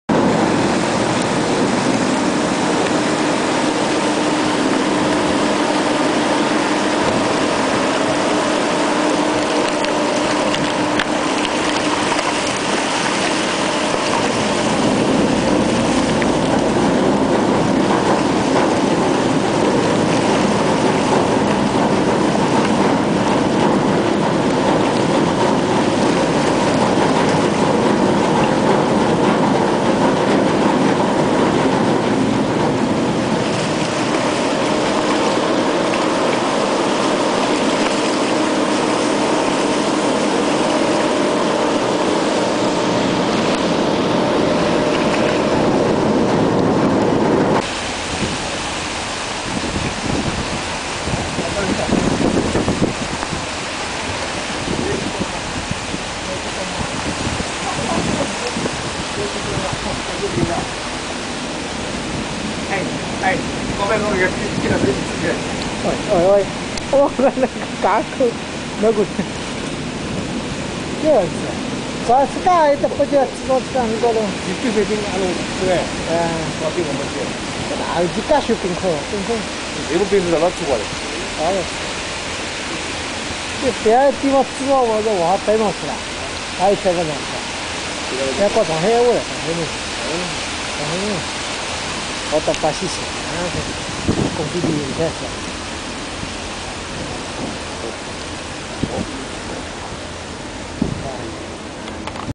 Near by Vernadski Station,Antarctica 26.12.2008
Ukrainian Antarctic station "Akademik Vernadsky" (ex "Faraday") started on February 6, 1996. The station is mainly meteorogical and geophysical observatory with introducing lasts years new researches in the ornithology, hydrology and human medicine.
New equipment has been installed at Vernadsky: high sensitive magnetometers to send the data in quasi-online to INTERMAGNET satellite system, the Shuman resonance receiver to monitor thunderstorm activity on the Earth.
The station is located on Galindez Island of Argentine Islands archipelago (65° 15 S, 64° 16 W) about 5 miles West of the Antarctic Peninsula. The coldest temperature:
-43,3 °C, the warmest: +11,8 °C.
About 300 days per year it is overcast and snowing.